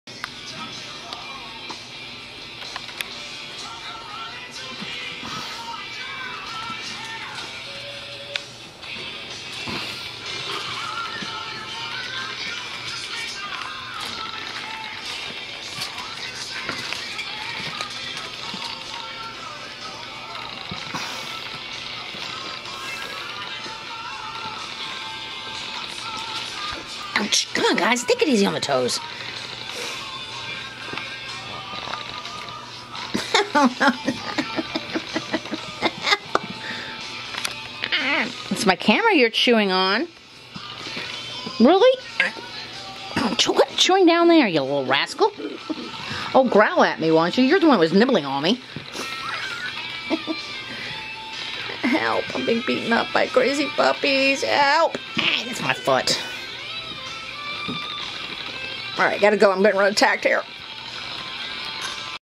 Wild Bills Side Show Litter 4 1/2 weeks old attack pups
Golden Retriever puppies at play and in attack mode.